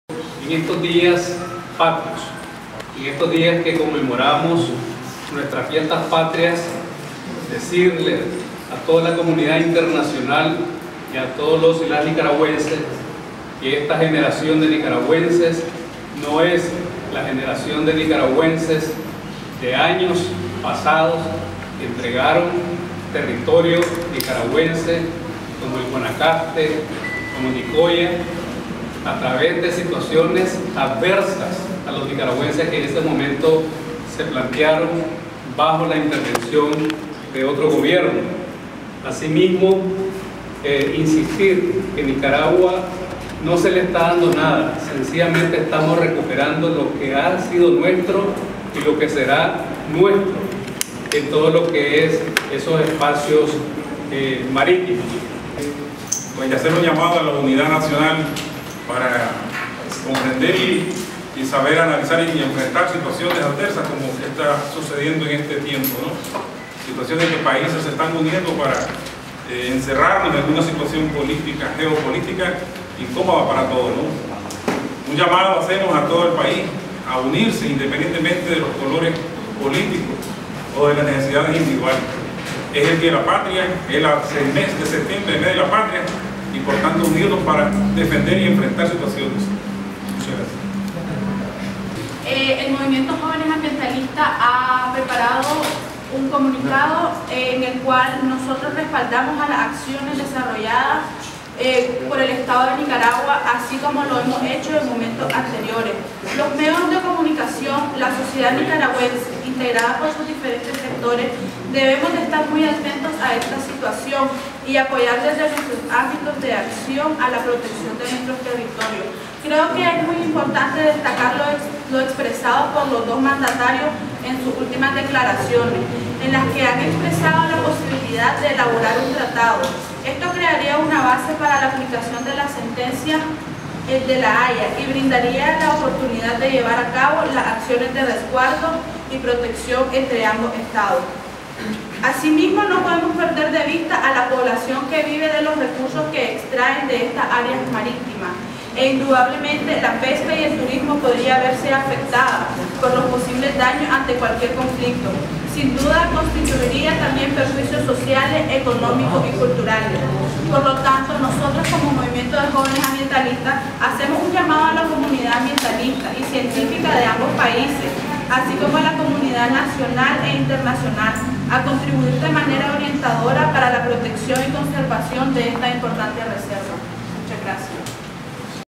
Ambientalistas piden a Colombia acatar fallo
Ambientalistas de Nicaragua, comandados por el Foro Nacional de Reciclaje, expresaron su apoyo a la posición del Gobierno en la defensa de la soberanía, y demandaron a Colombia cumplir el fallo de la Corte Internacional de Justicia que definió los límites entre ambos países en el Caribe. MANUEL ZAPATA / END